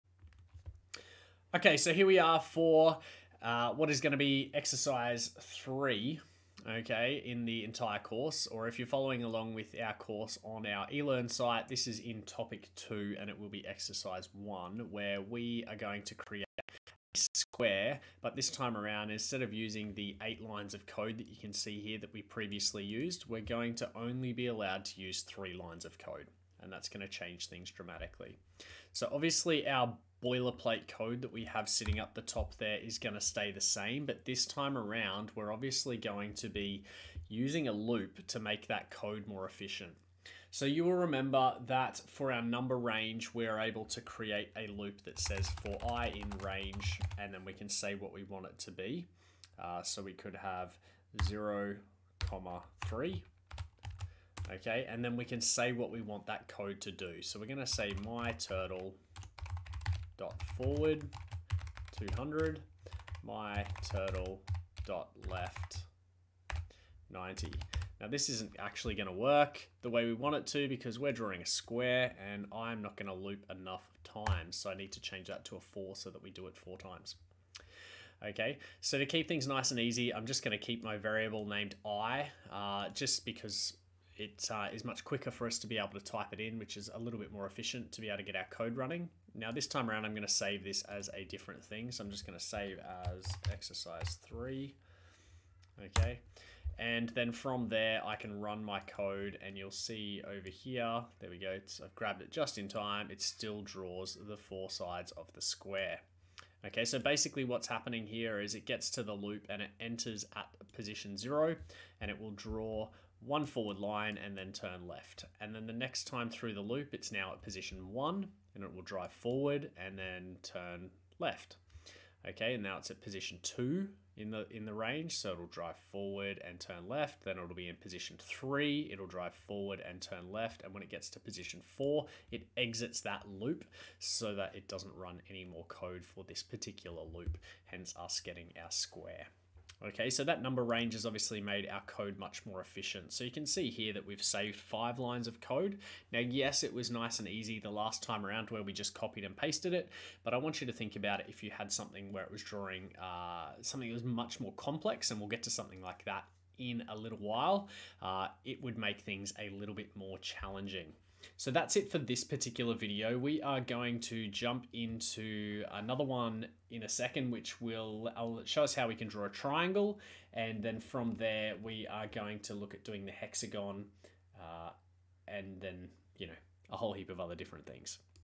Python Turtle Basics - Drawing a Square using a Loop
In this video we quickly make our previous code very efficient. Specifically we look at how the original code for our square can be improved so that there are only 3 lines of code instead of 8.